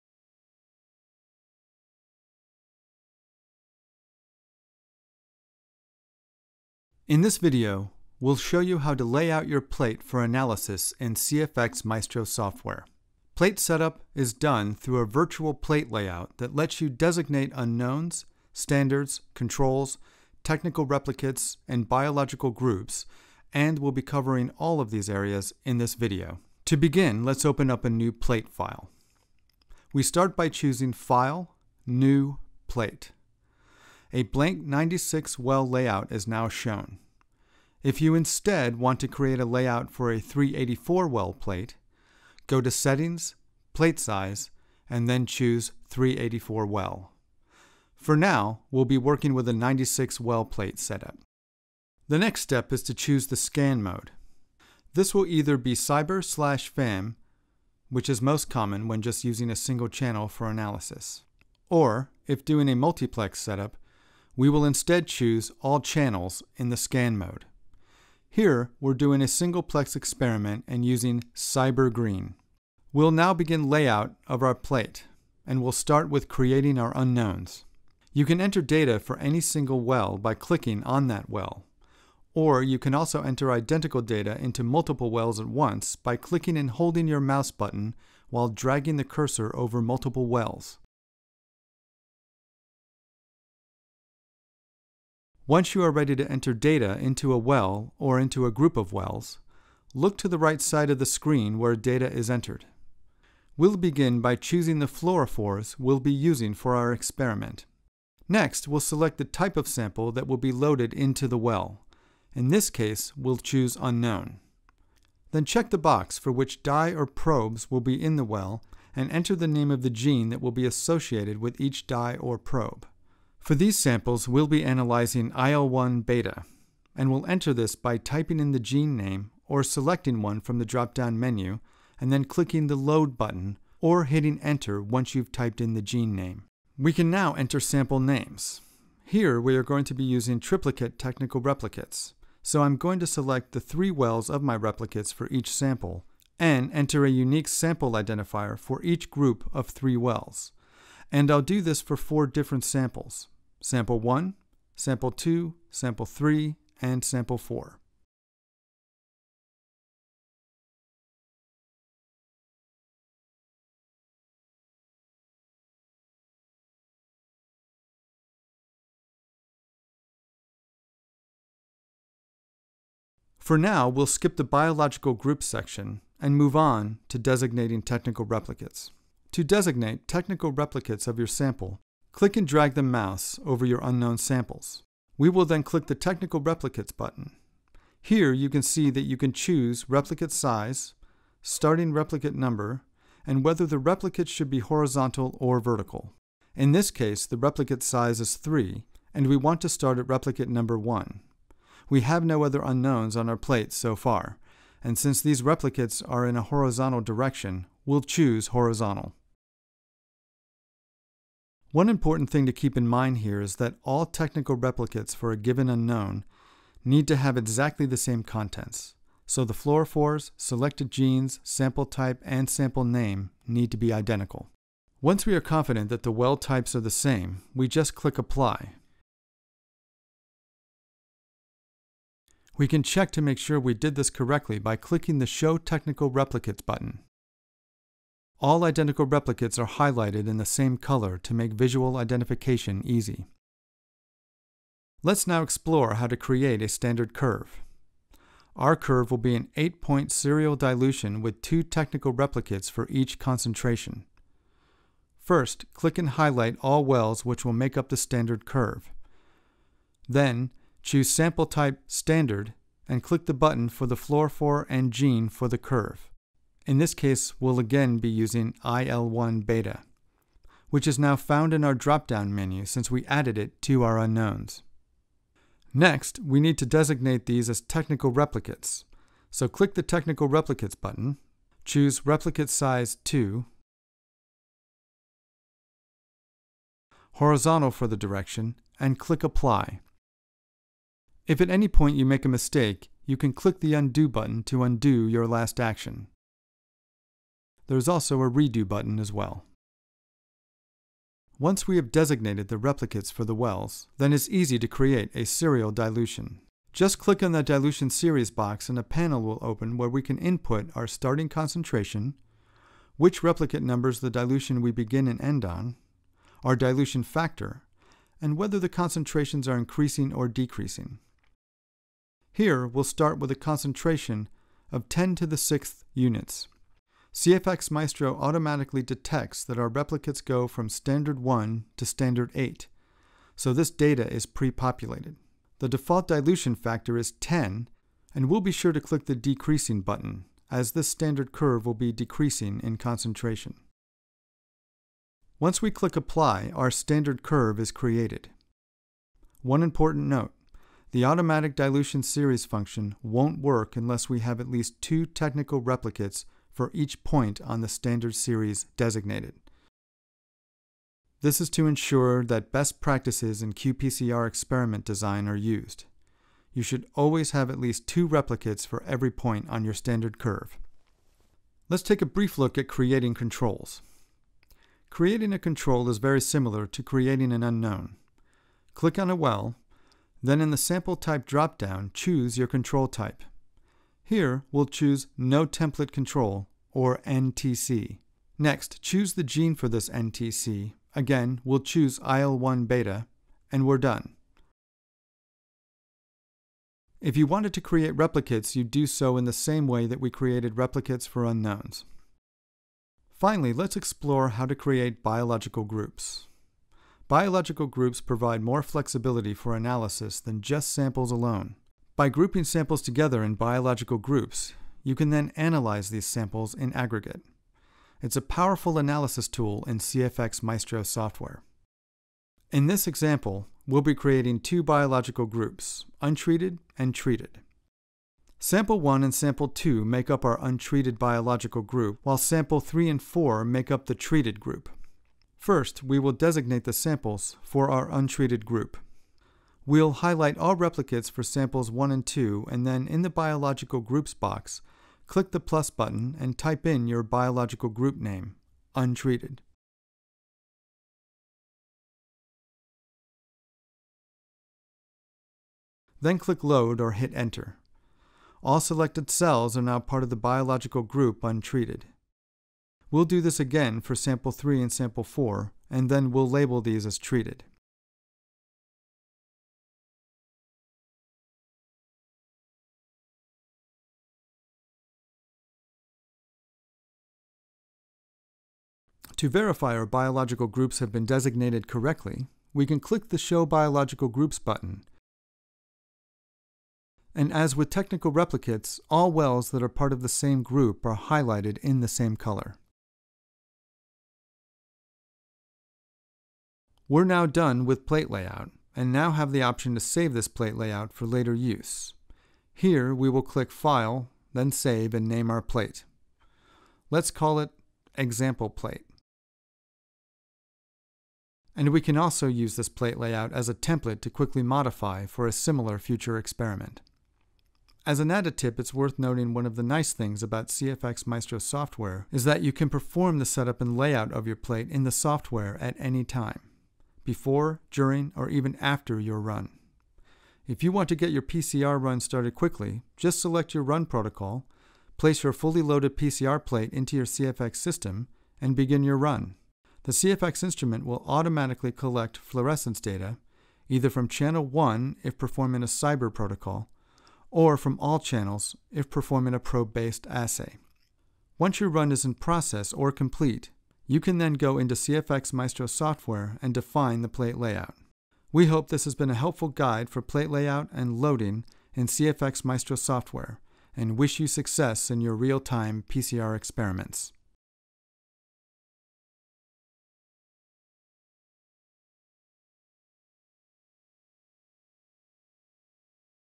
qPCR Analysis with CFX Maestro™ Software: Plate Setup
This tutorial will explain how to perform plate setup for your qPCR experiments in CFX Maestro Software.

 Covered topics include:
 -Creating unknowns
 -Designating technical replicates
 -Creating biological groups
 -Easy standard curve creation
 -Setting up controls

 CFX Maestro Software provides deatiled qPCR analysis including:
 -Gene Expression analysis
 -Multiple data visualization modes
 -Statistical analysis with t-tests and ANOVA
 -High-resolution graph export

 CFX Maestro Software is also available in a Security Edition for 21CFR part 11 needs, and in a Mac version for data analysis.